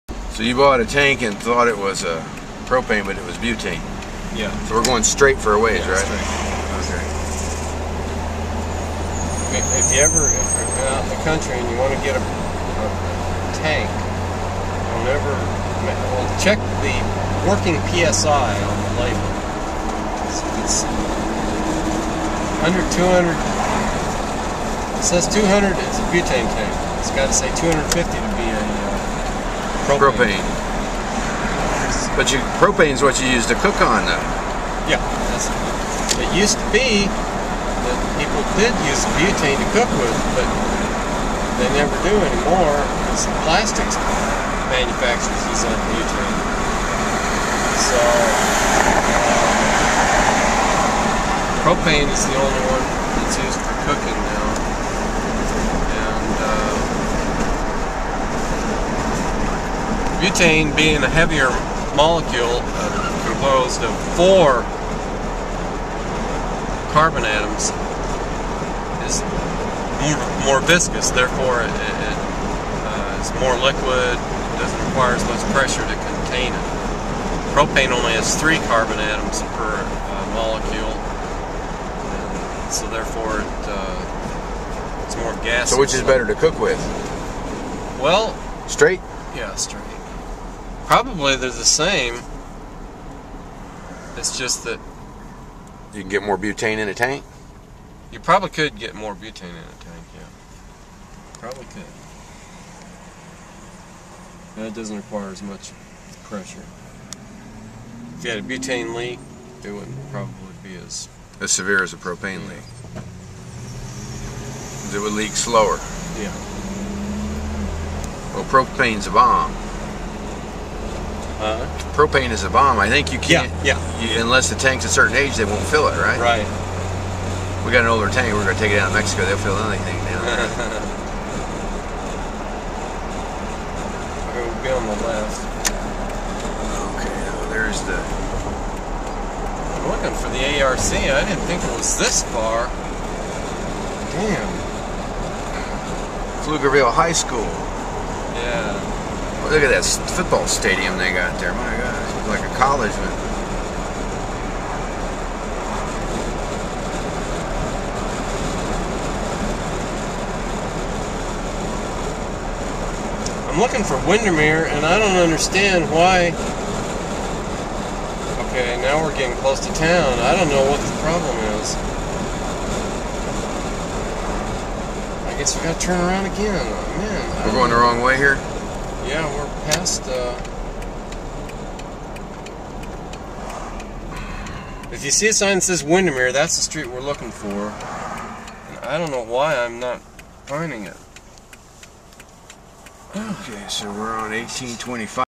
Propane vs. Butane working PSI; butane is more viscuous and leaks slower
200 is butane; 250 is propane...don't use butane anymore to cook; 5 atoms vs. 3 atoms